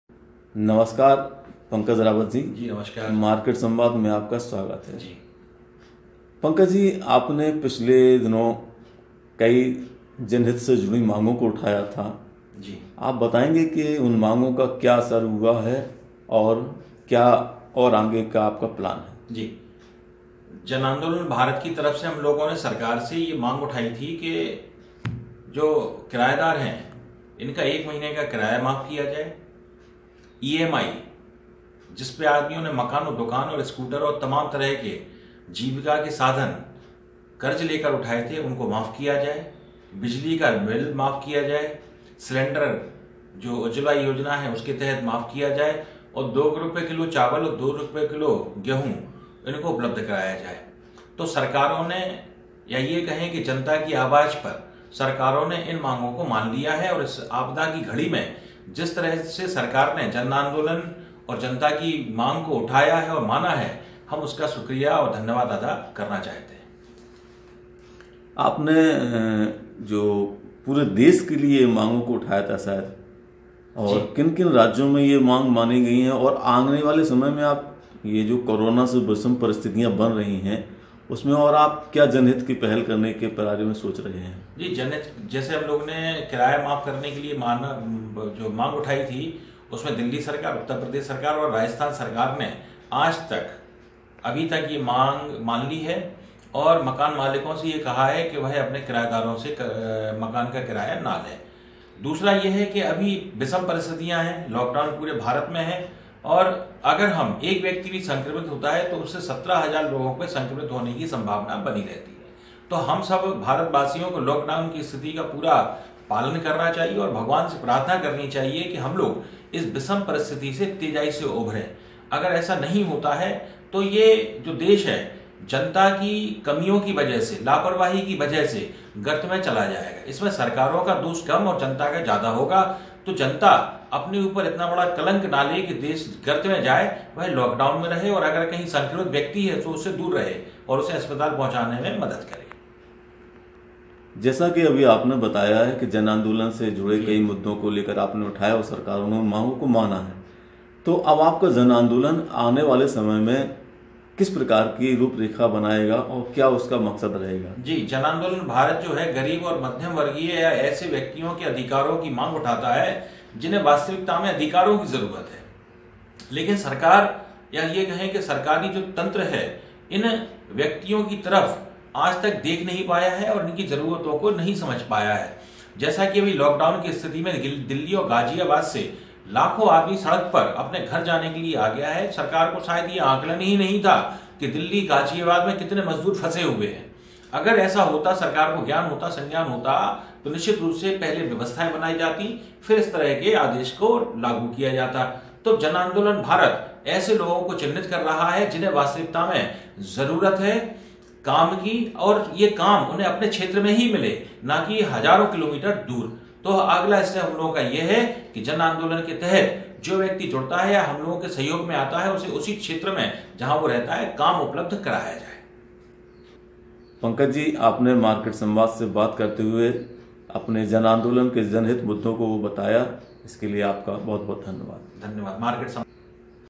जन आंदोलन के किरायदारों की किराया माफ़ी पर झुक गयी देश की कई सरकारें
जन आंदोलन भारत के संयोजक पंडित पंकज रावत ने कोरोना वायरस के चलते देश के विभिन्न राज्यों में रहने वाले किराएदारो का मार्च अप्रैल का किराया और गरीबों को गैस सिलेंडर देने की आवाज उठायी। उनकी ये मांग शोसल मीडिया पर खूब वायरल हुई। इसके बाद यूपी, दिल्ली आदि राज्यो में सरकारों ने किरायदारों को राहत देने वाले कदम उठाये है।